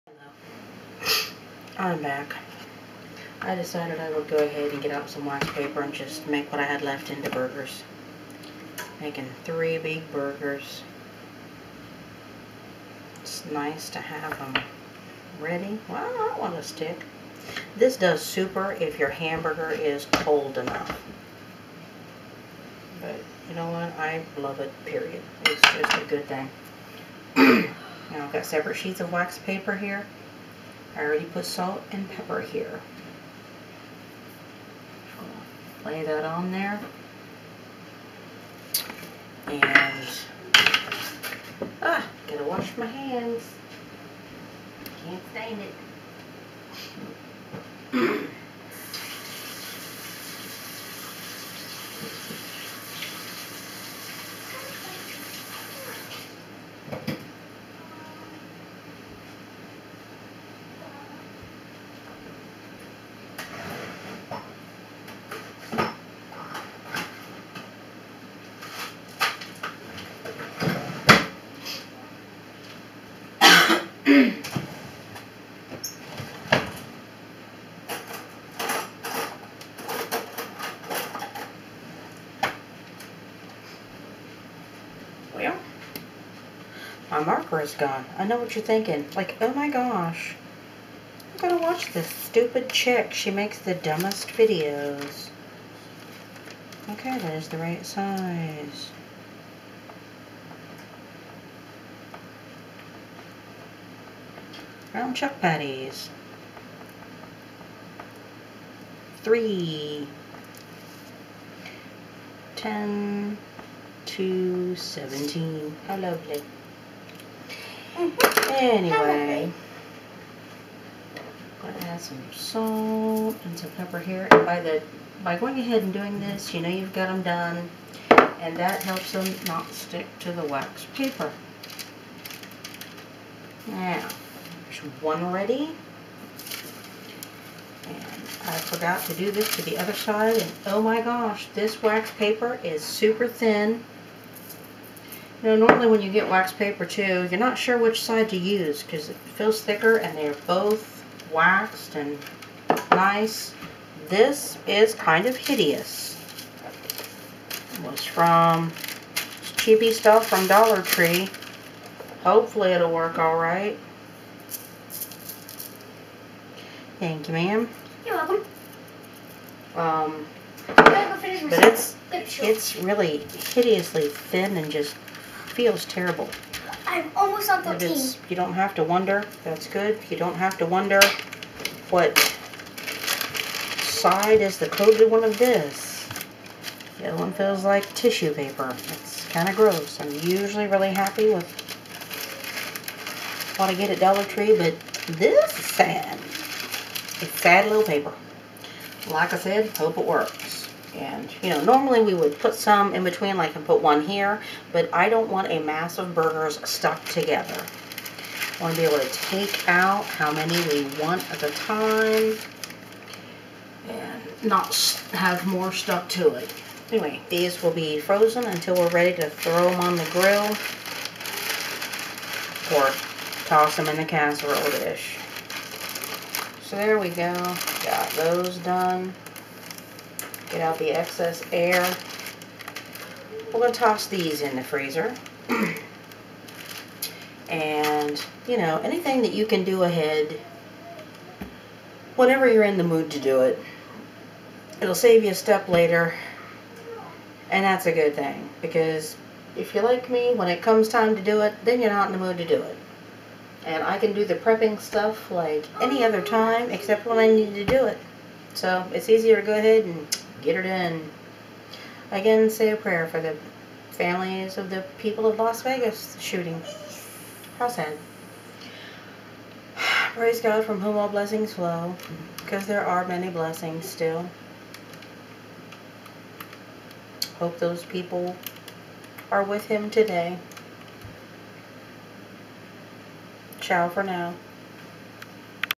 Freezer Burger Prep.
Left over ground chuck--patties for the freezer.